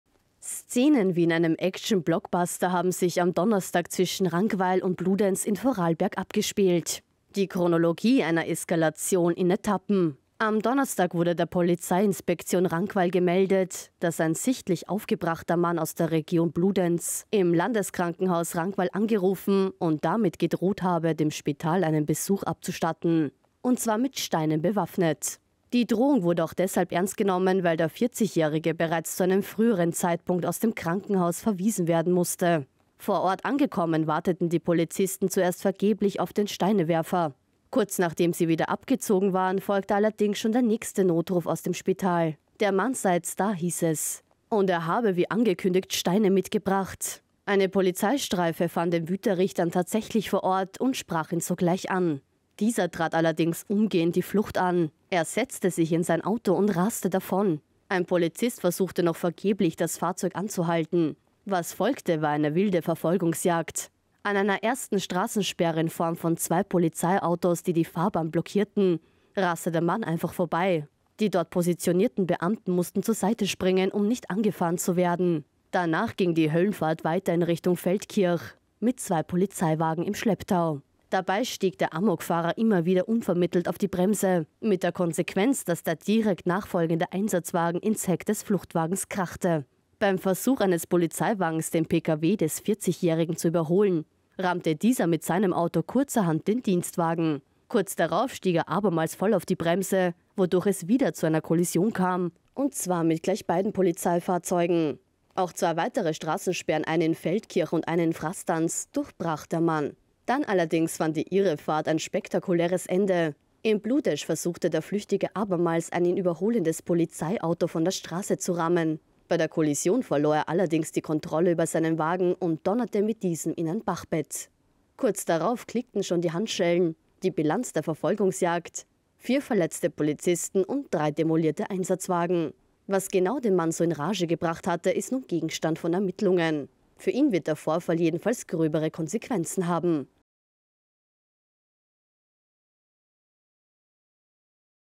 Vier Polizisten bei irrer Verfolgungsjagd verletzt | krone.tv NEWS
Szenen wie in einem Action-Blockbuster haben sich am Donnerstag zwischen Rankweil und Bludenz in Vorarlberg abgespielt. Ein 40-jähriger Mann hatte erst vor dem Landeskrankenhaus Rankweil Krawall geschlagen und war anschließend mit seinem Auto davongerast. Es folgte eine Verfolgungsjagd, bei der gleich mehrere Polizisten verletzt wurden.